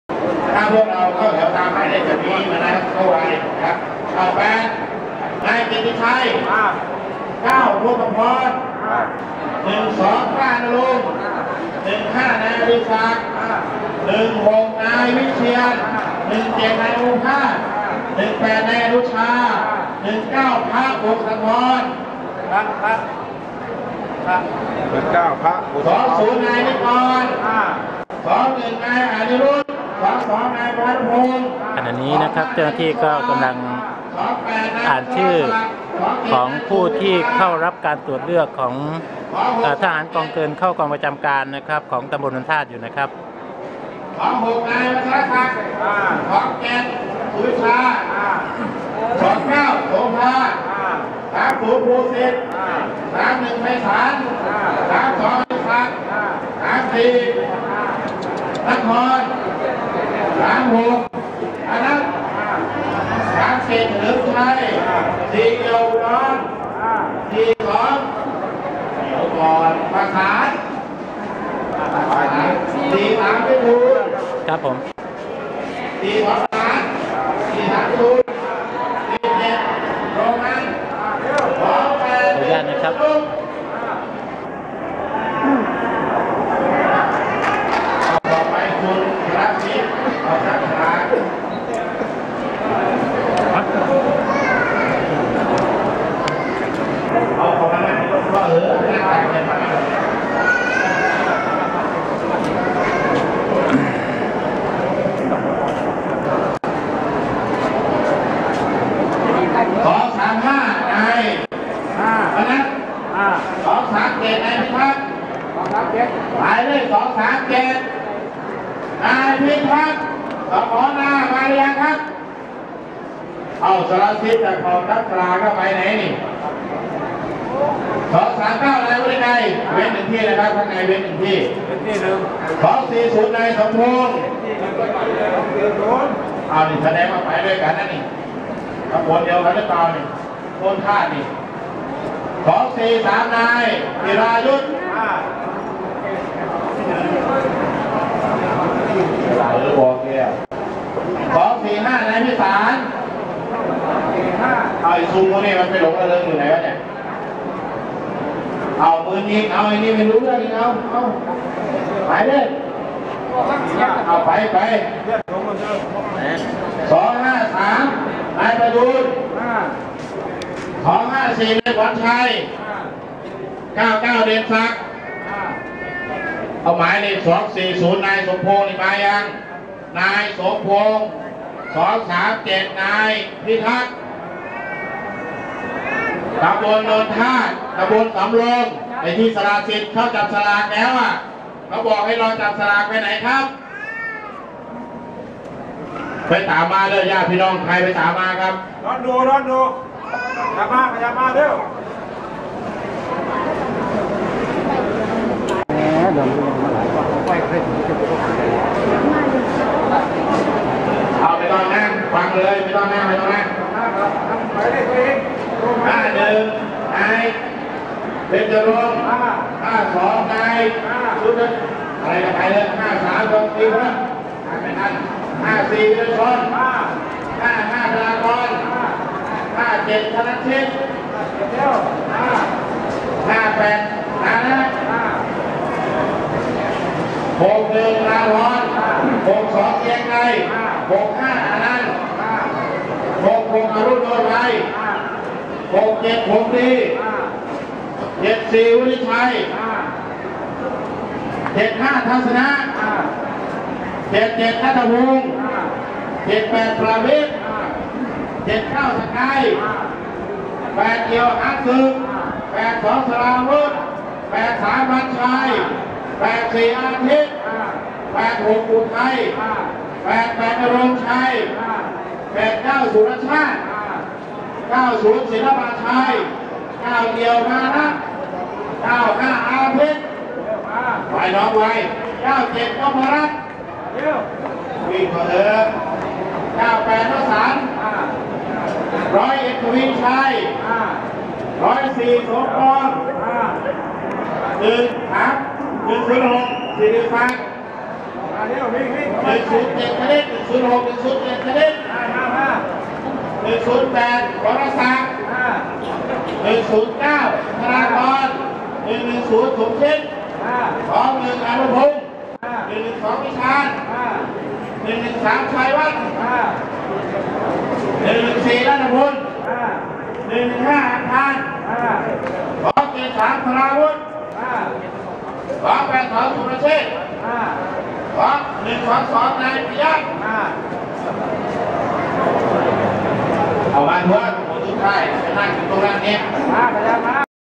การตรวจเลือกทหาร ปี 2556 part 3
อำเภอหนองสองห้อง ได้ทำการตรวจเลือกทหารกองเกินเข้ากองประจำการ ประจำปี 2556 ในวันที่ 2-3 เมษายน พ.ศ.2556 ตั้งแต่เวลา 07.00 น.เป็นต้นไป ณ หอประชุมอำเภอหนองสองห้อง อ่านรายละเอียดเพิ่มเติมคลิก